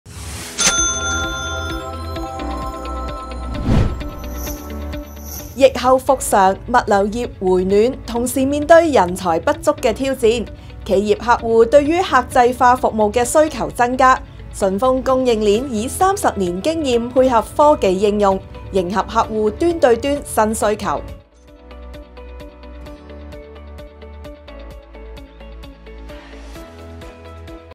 【ET贏商有計】順豐供應鏈提升自動化能力 提供客製化方案迎市場需求
疫後復常，物流業回暖

同時面對人才不足的挑戰

企業客戶對客制化服務需求增加

順豐供應鏈以30年經驗配合科技應用

迎合客戶端對端新需求